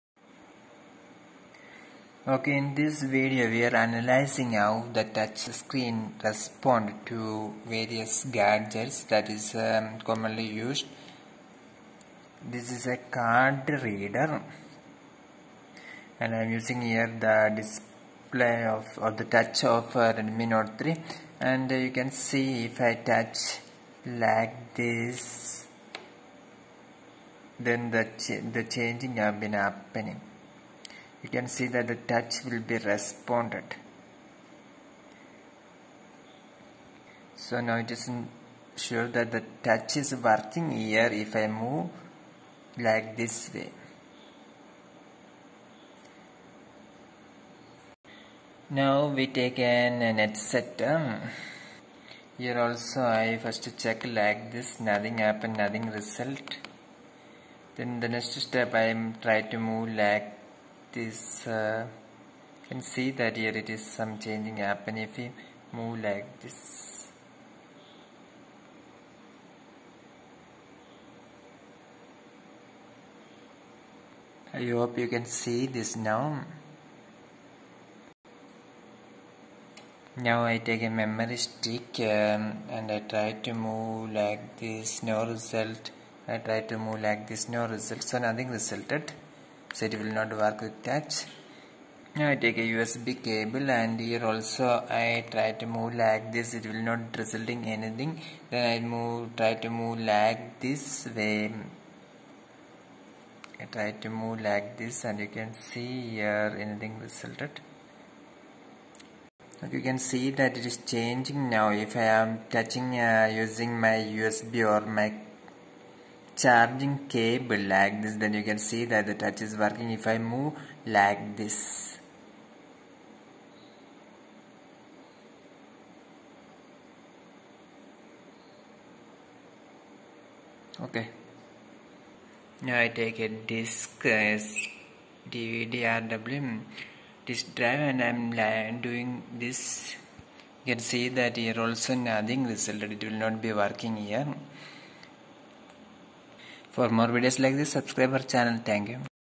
How mobile touch respond to card reader, usb cable, net setter, memory card
How mobile touch respond to card reader, usb cable, net setter, memory card  etc. is checked in this video.